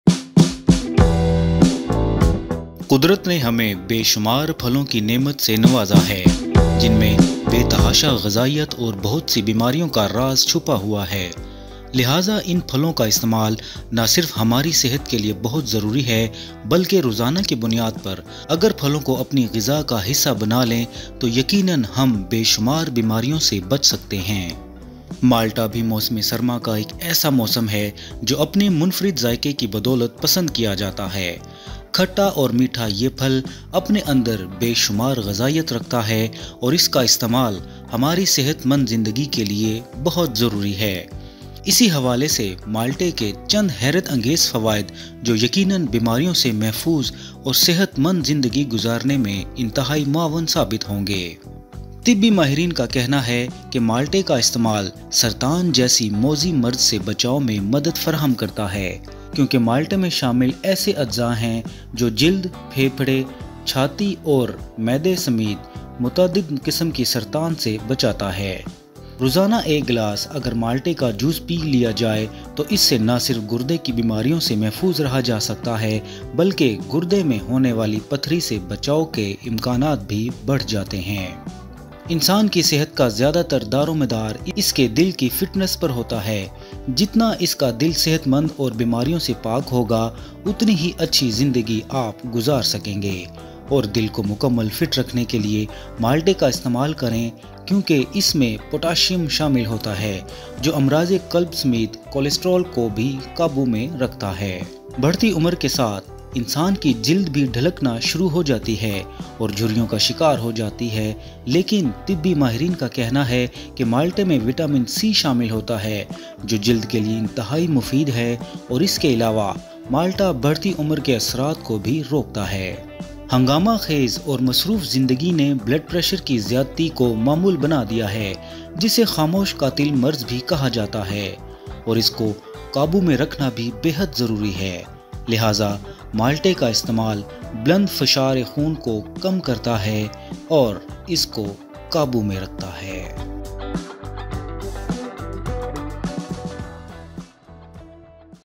Benefits Of Oranges - Kinu Khane Ka Fayde/Fawaid | National News Nama
Benefits Of Oranges - Kinu Khane Ka Fayde/Fawaid - Best Time To Eat Oranges - Body Detoxification

Oranges can be had not only as a snack but also as a major recipe ingredient in various dishes. One cup of oranges contains only 85 calories, making this a nutrient-dense, low-calorie food. The same serving also contains 4.3 grams of dietary fiber. This compound aids in weight loss by promoting regular bowel movements and helping you feel full for longer periods of time.

Oranges are an excellent source of vitamin C. One orange offers 116.2 percent of the daily value for vitamin C. Good intake of vitamin C is associated with a reduced risk of colon cancer as it helps to get off free radicals that cause damage to our DNA. Oranges are a great way to body detoxification. The Best time to eat oranges in the day as a snack or after a meal.